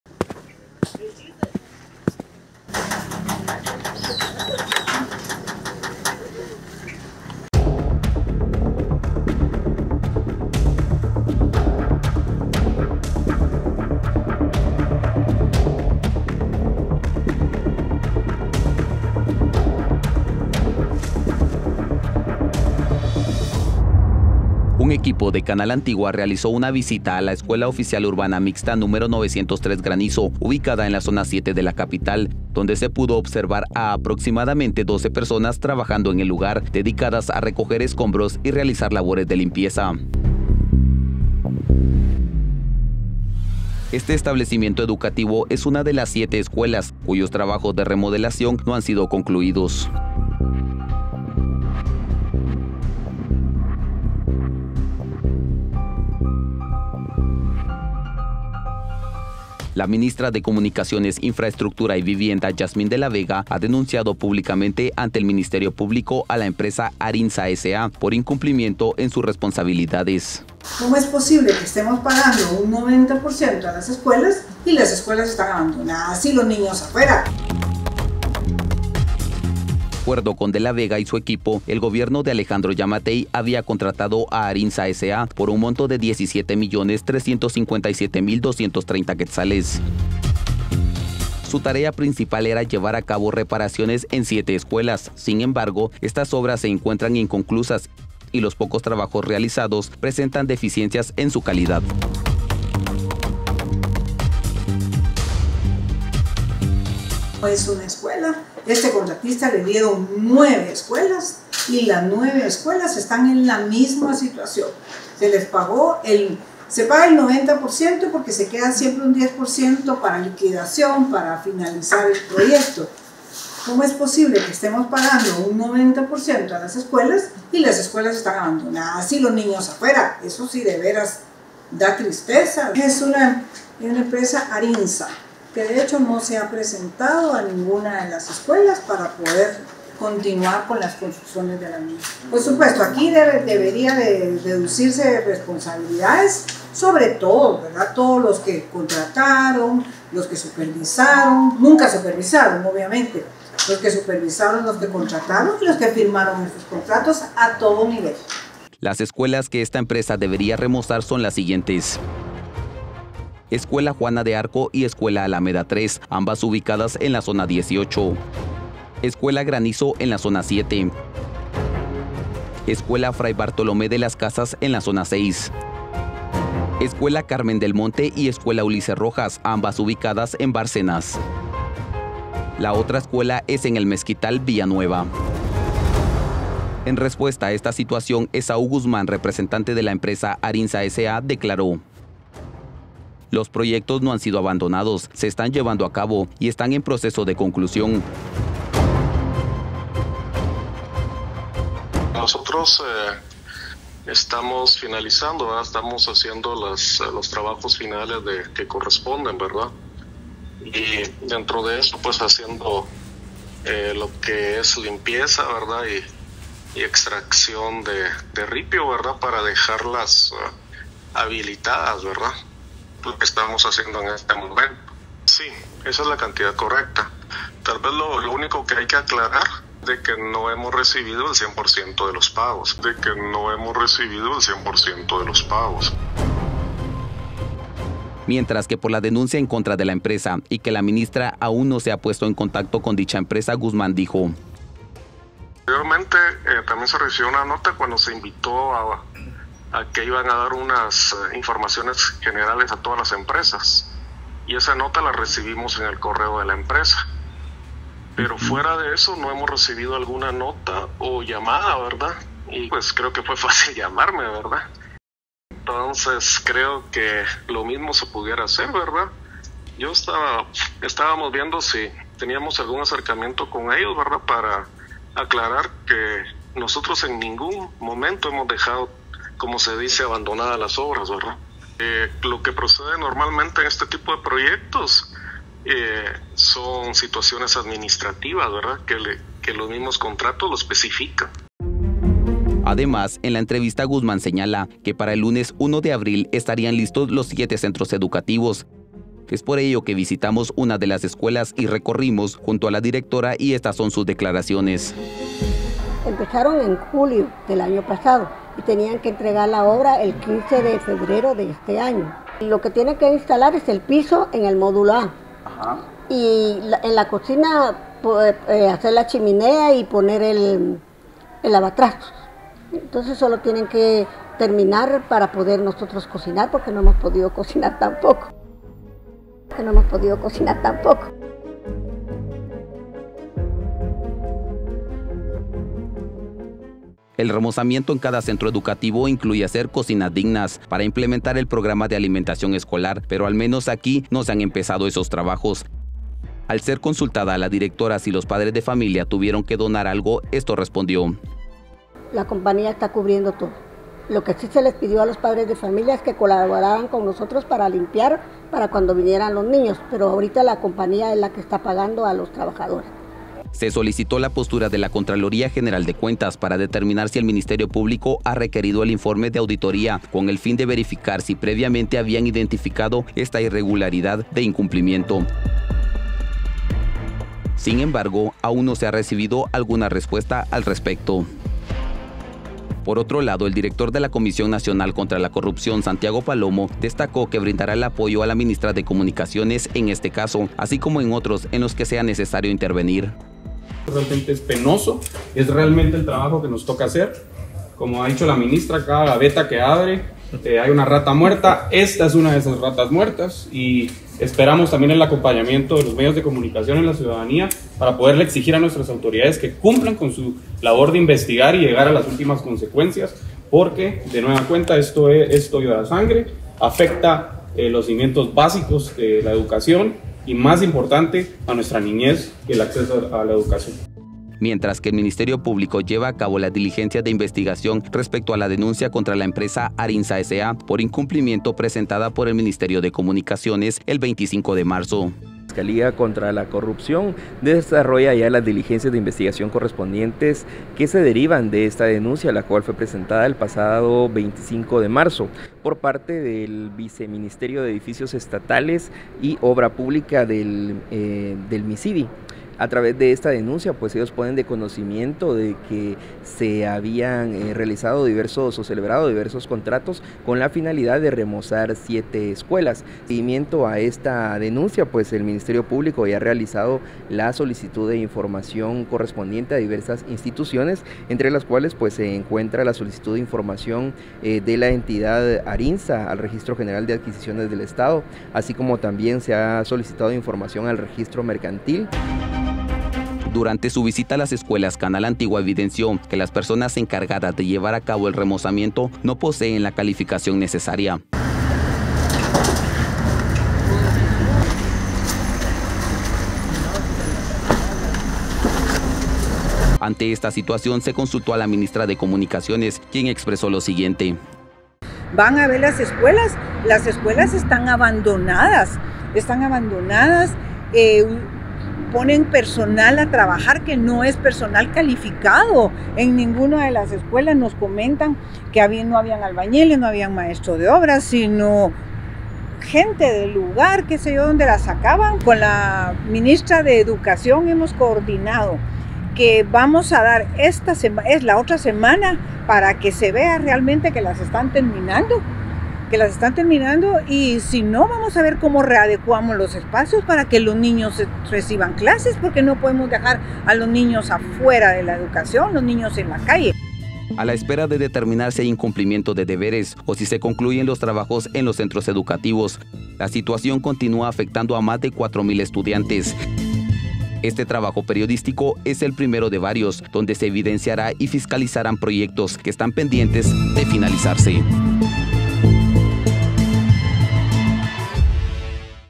Reportaje Especial: contratos incumplidos para remozamiento de escuelas
Canal Antigua le dio seguimiento a la denuncia que presentó la titular de la cartera de Comunicaciones, Infraestructura y Vivienda (CIV) Jazmín  de la Vega, a la empresa Arinsa, S. A., por incumplimiento.